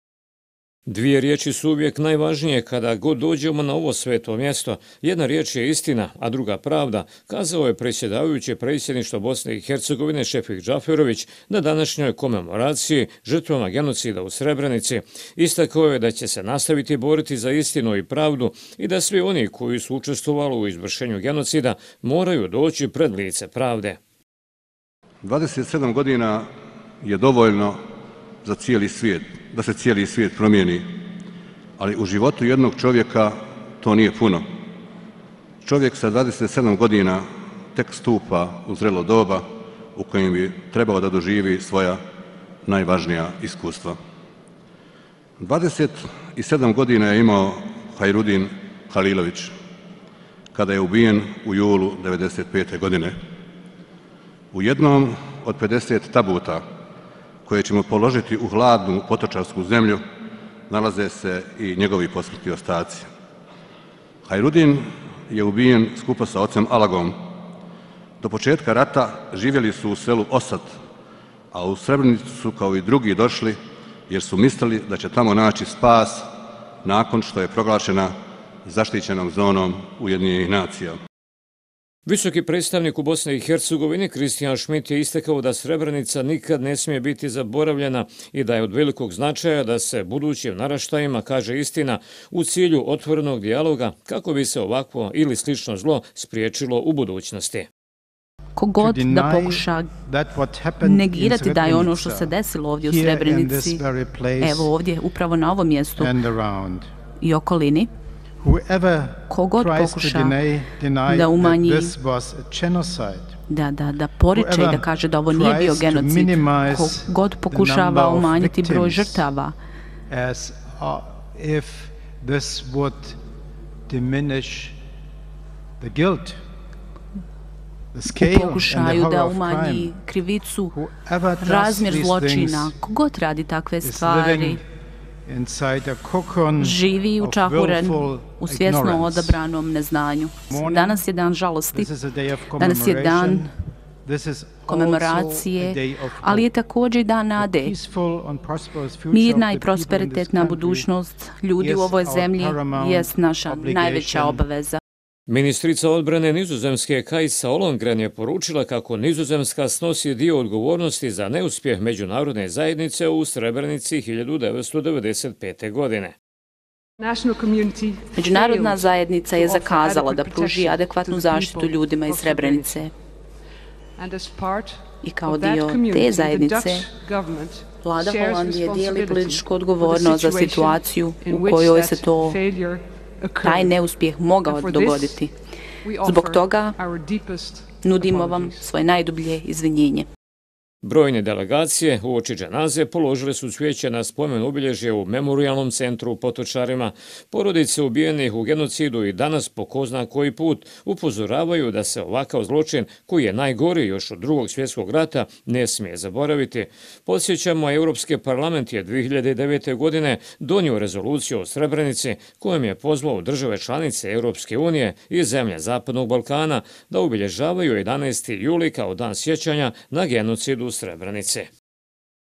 KLANJANA DŽENAZA I OBAVLJEN UKOP 50 ŽRTAVA GENOCIDA U SREBRENICI
Vječni smiraj u Memorijalnom centru Potočari ove godine je našlo 50 žrtava genocida, počinjenog u Srebrenici 1995. godine.